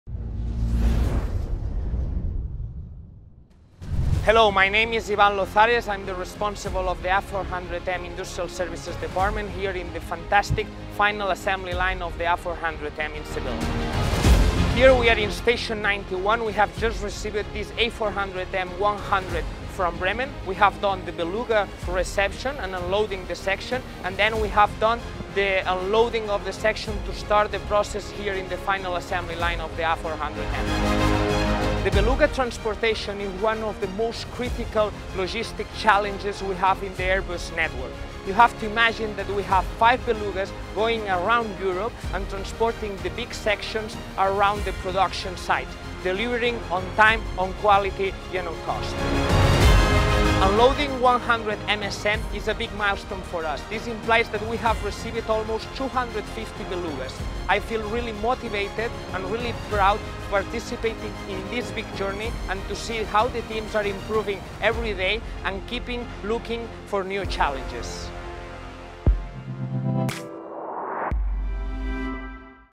14. How to build an A400M - Beluga delivers
Airbus’ oversize cargo airlifter – affectionately known as the Beluga – arrives at Station 91 of the Spanish final assembly line, carrying the fuselage for A400M MSN 100. In the next video, the A400M gets a nose job…